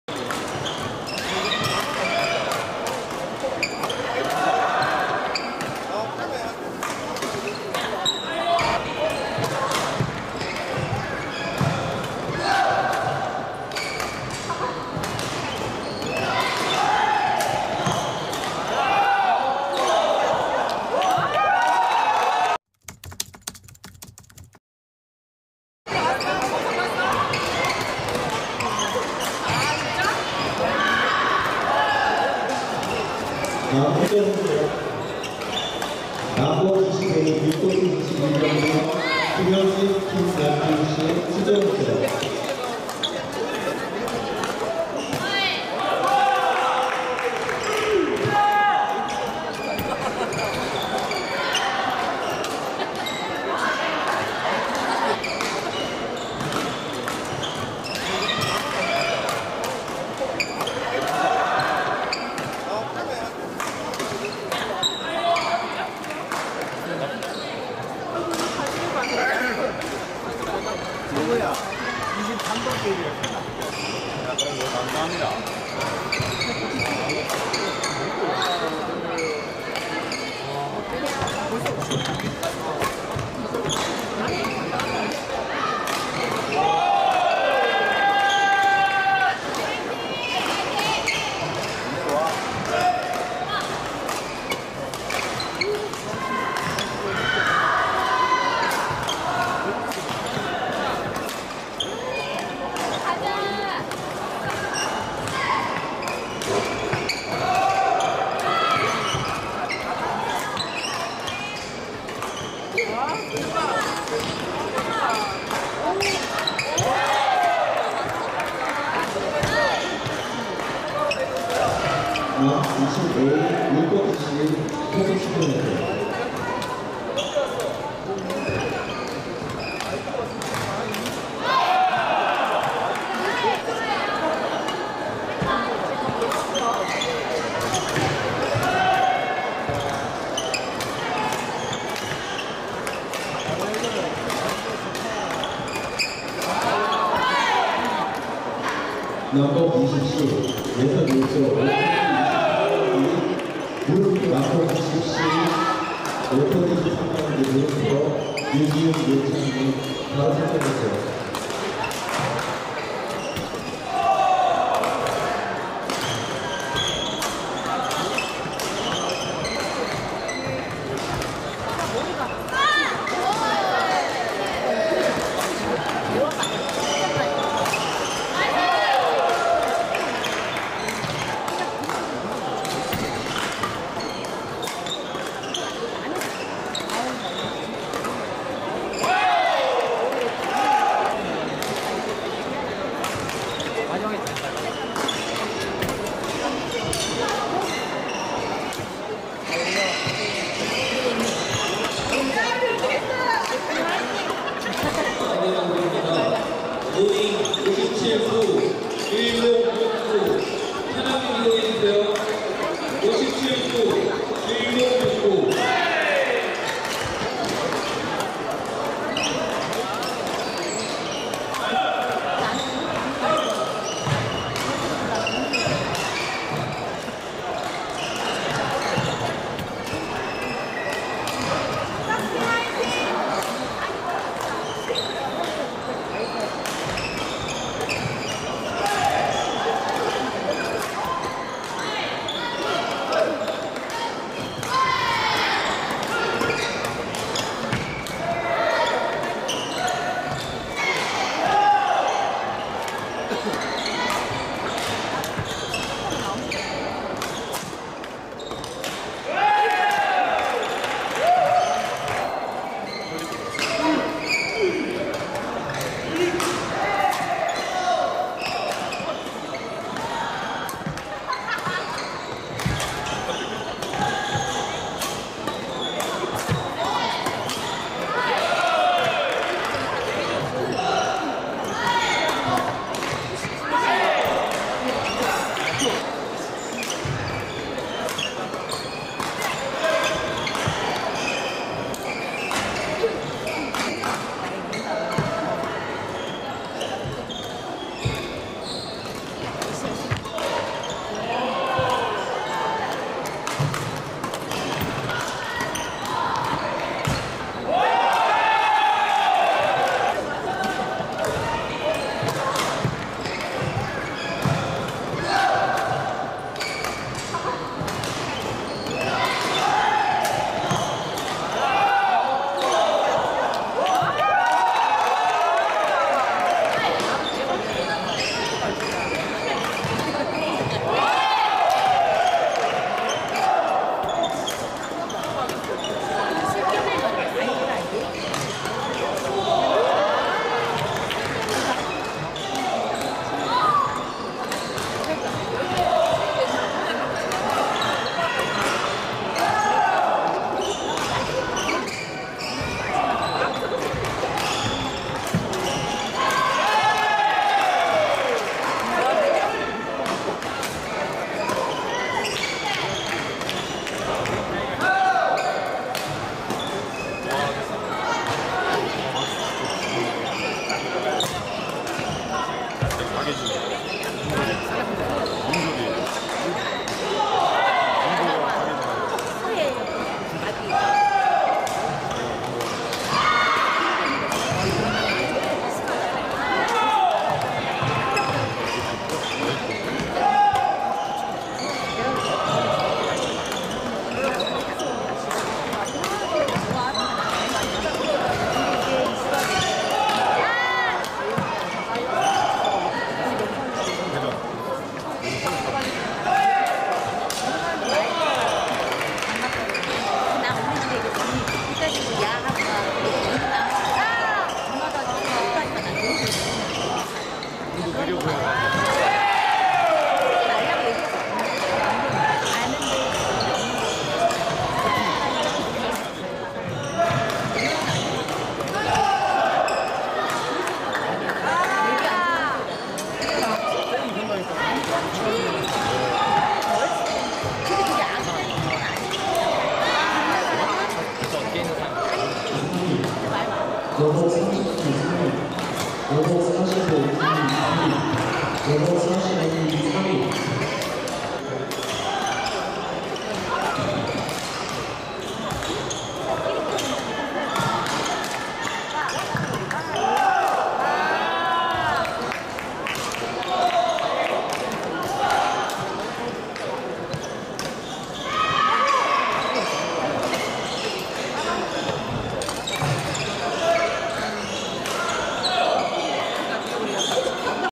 배드민턴 여복최강자들의 경기 한번 보시죠. (윤솔이,박예진,박지원,박진희)
제2회 민턴데일리엔 OPEN 여복20A 박예진 | 윤솔이 vs 박지원 | 박진희

대회 : 제2회 민턴데일리엔 전국 OPEN 배드민턴 대회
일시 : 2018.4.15
장소 : 충주호암체육관
선수
팀명